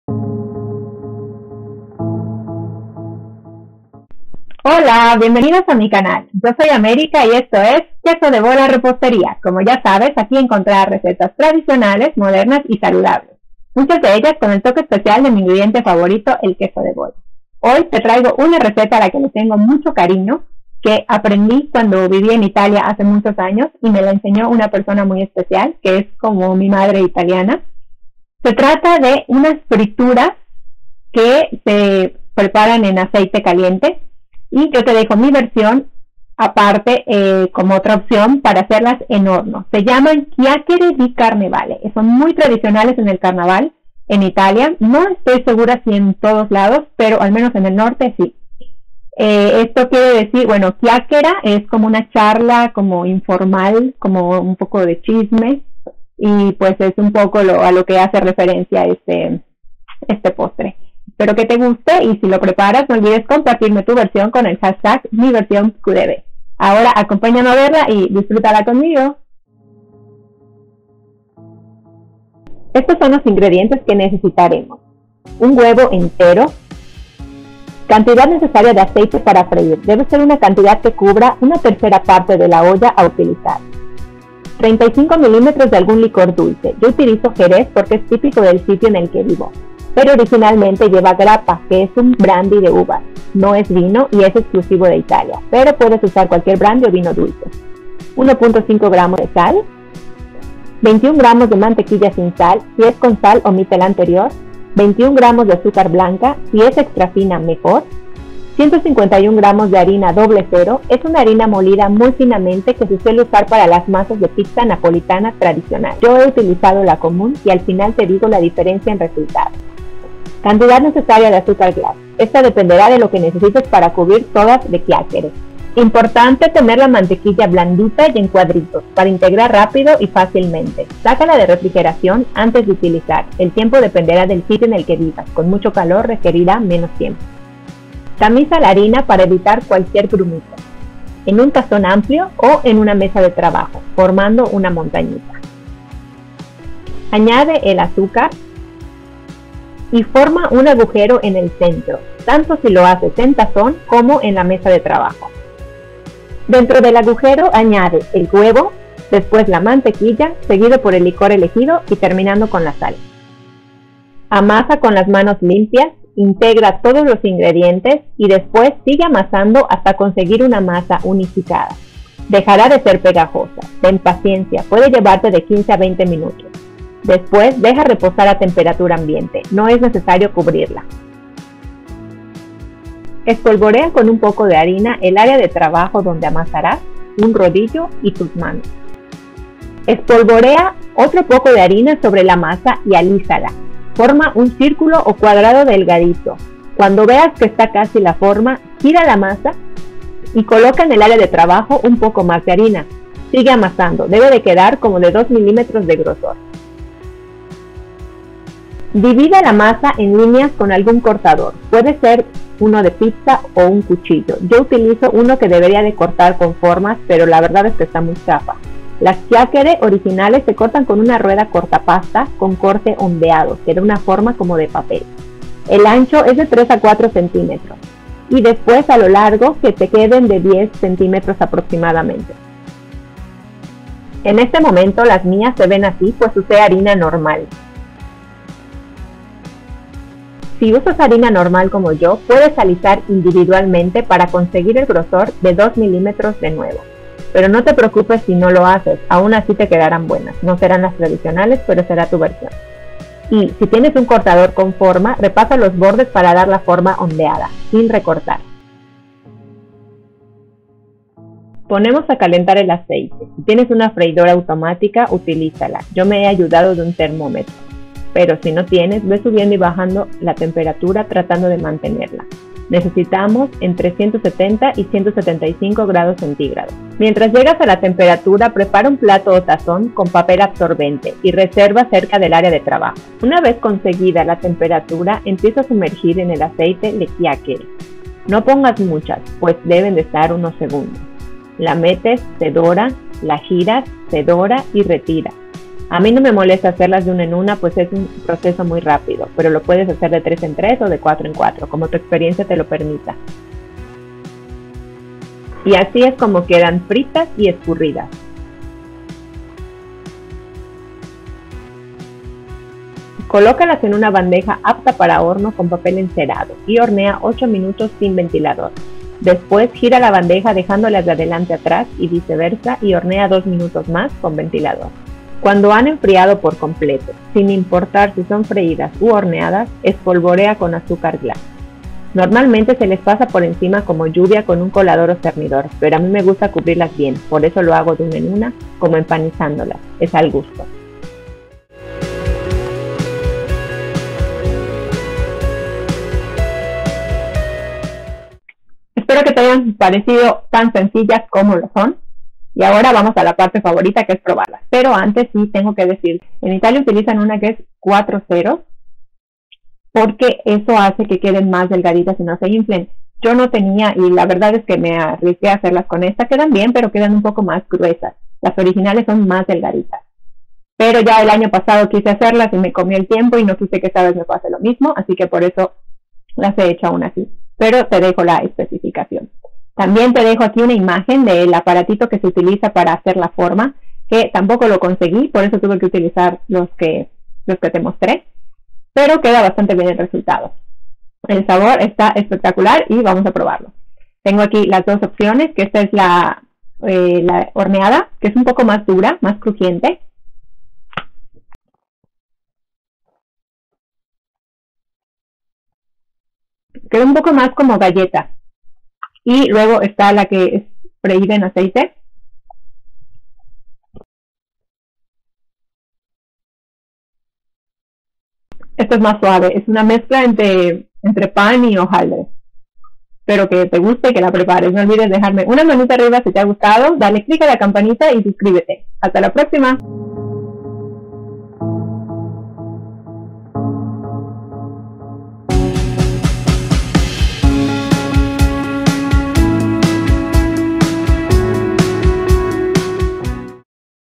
CHIACCHIERE DI CARNEVALE | REPOSTERÍA TRADICIONAL | FRITAS Y AL HORNO | RECETA ITALIANA | #50
Me adelantó un poquito porque ya tengo programadas las recetas de todo el mes y parte de marzo y no quería dejar de compartir esta receta tan especial para mi.

Postre típico en Italia, para el carnaval; te recomiendo prepararlo para celebrar con tus amigos y familiares, te aseguro que los sorprenderás.
La receta que te dejo es la de Pinuccia, mi madre italiana, que es una estupenda cocinera; claro que como siempre tengo mi versión porque es inevitable creo que siempre por mucho que tengas una receta y quieras seguirla al pie de la letra por una u otra razón terminas cambiando o ajustando cosas.
¿A ti también te ocurre?

Te dejo dos maneras de preparar, la tradicional fritas en aceite y horneadas.
Mas detalles en el Blog.

Enlace a la sección Ingredientes y utensilios para ver el cortador en rodillo y la harina 00:

Si te gustó comparte, con quien creas que disfrutará esta receta, ESTE ↴ ↴ enlace:

Prepáralo y envíame una foto, me dará mucha ilusión verlo y utiliza el hashtag:

EDICIÓN: América García

* Ingredientes para 30 pzas de 3 x 10 cm:
1 tza + 3 cdas | 151 g   Harina de trigo 00, o una muy fina
1 cda + 1/2 dta | 21 g   Azúcar blanca, mejor si es extrafina
1 cda + 1/2 dta | 21 g   Mantequilla sin sal a temperatura ambiente
1 pza | 55 g                     Huevo L
2 cdas + 1 cdta | 35 ml Grappa, brandy o licor dulce
1/4 cdta | 1.5 g               Sal
C/N                                   Aceite vegetal para freír
C/N                                   Azúcar glas, velo, flor o impalpable

* ¡QuéTips!
-  La harina 00 es una harina muy refinada, puedes utilizar la más fina que encuentres o utilizar la normal, pero te quedarán un poco más gruesas como me pasó a mí. De todos modos, en la sección "Ingredientes y utensilios” del Blog puedes encontrar en donde comprarla.

- El azúcar extrafino permite que se integre mejor y ocupe menos volumen en la masa, lo que le permitirá mantener su delgadez, pero queda aún con la normal.

- La grappa es un tipo de brandy de uva exclusivo de Italia, se puede utilizar ese, otra variedad de brandy o algún licor dulce, yo he utilizado jerez, que es una bebida común en España, donde vivo.

- Para hacer la forma tradicional se utiliza una rueda con rodillo rotulador, en la sección "Ingredientes y utensilios” puedes encontrar las opciones de compra.

- Dependiendo de la zona suelen hacerlas como lazo, con un nudo o con un orificio alargado en el interior, como si fuera una dona, pero rectangular.

- Puedes hacerlas fritas de la manera tradicional, su textura es entre un bizcocho y un hojaldre; o bien puedes hornear, pero su textura quedará más crujiente, como una galleta. Ambos son muy ricas.

-  Se conservan de 3 a 4 días en un tóper hermético, o en una bolsa bien cerrada.

- ¡DISFRUTA!